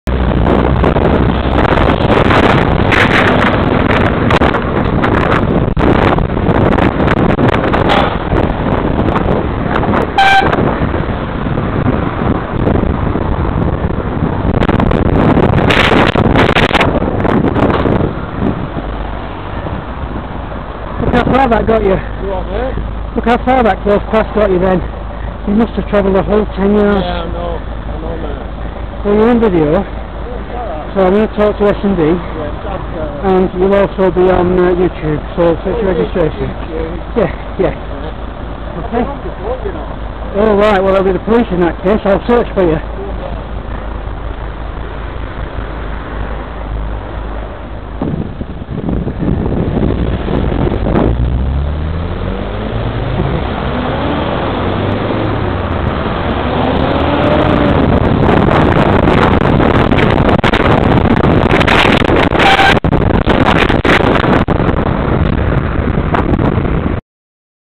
YE02WXV - Taking The P155 (S & D Private Hire)
This private hire driver is truly an arrogant, ignorant git. I will be speaking to his company, the licensing authority and the police about his behaviour when driving vehicle YE02WXV, a Ford Focus Zetec (Estate).

Time: 8.00am 25th June 2011
Location: Moses Gate. Bolton Road. Southbound carriageway.
Registration: YE02WXV
Company: S & D Private Hire
Bolton licence number: 0017